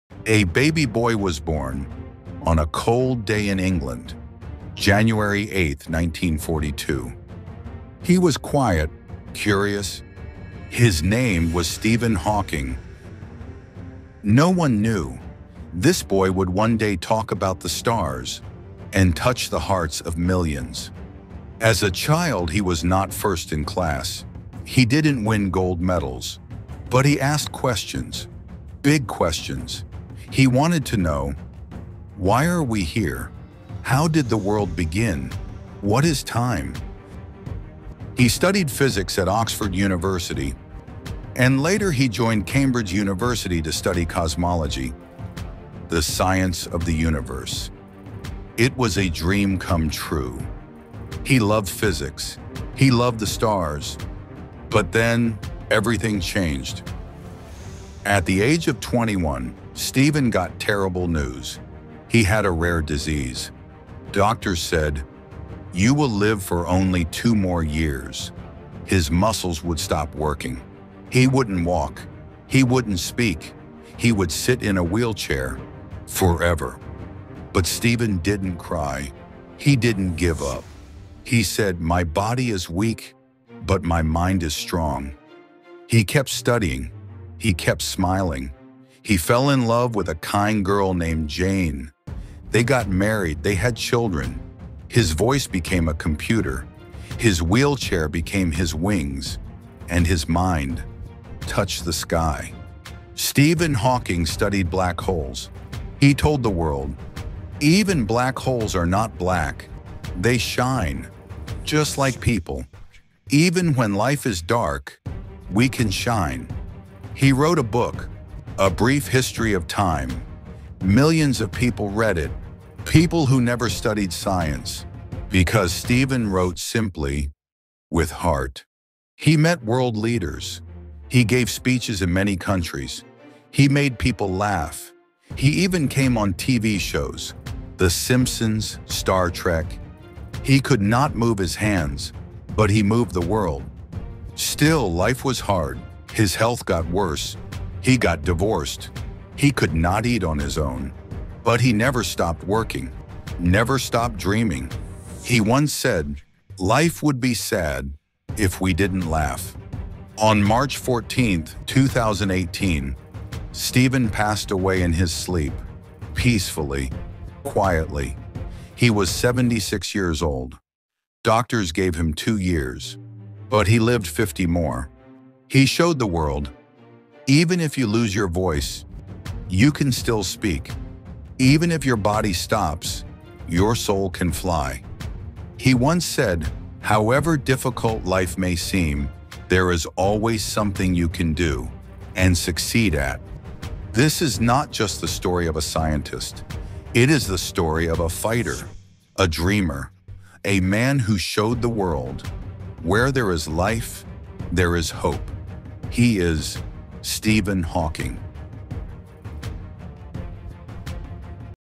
The Man Who Lost His Voice… But Changed the World | Stephen Hawking’s Story
This is the emotional, simplified story of Stephen Hawking — told in a way learners, students, and dreamers around the world can understand and feel.

From being diagnosed with ALS at age 21…
To becoming the world’s most famous scientist in a wheelchair…
Stephen showed us that even if the body is broken, the mind can fly.